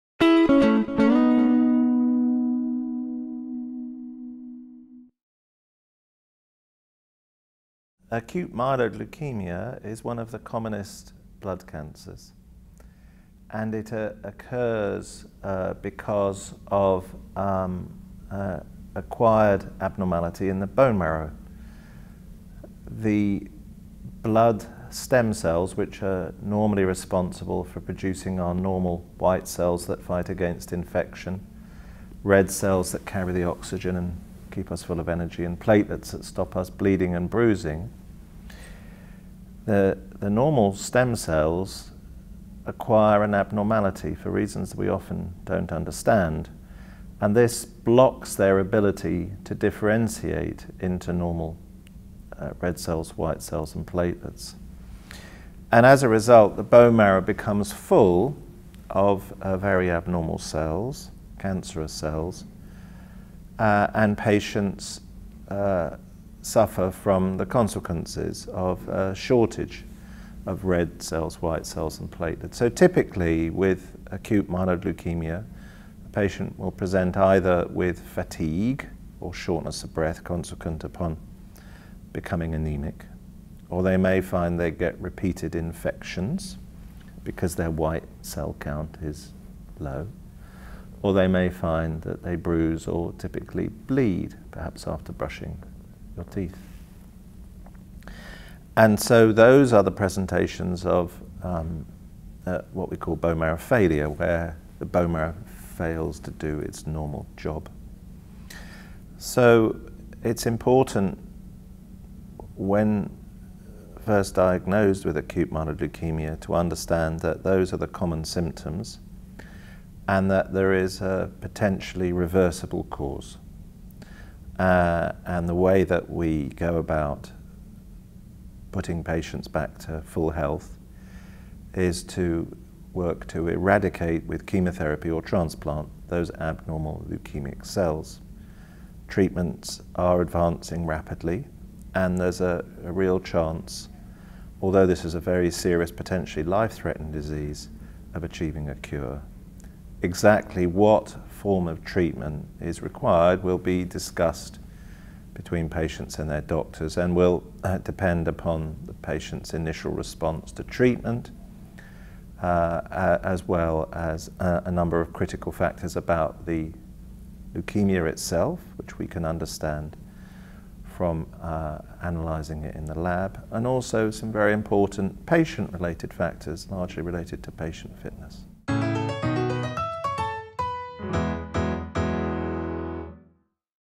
What is acute myeloid leukaemia (AML)?
Prof. Charles Craddock of the Queen Elizabeth Hospital in Birmingham, UK, describes what acute myeloid leukaemia is. In this video he describes the biological process that leads to the development of acute myeloid leukaemia (AML), how it presents in the form of signs and symptoms, and how it is normally treated.
Recorded at the American Society of Hematology (ASH) 2015 Annual Meeting in Orlando, FL.

This programme has been supported by Pfizer, through an unrestricted educational grant to the Patient Empowerment Foundation.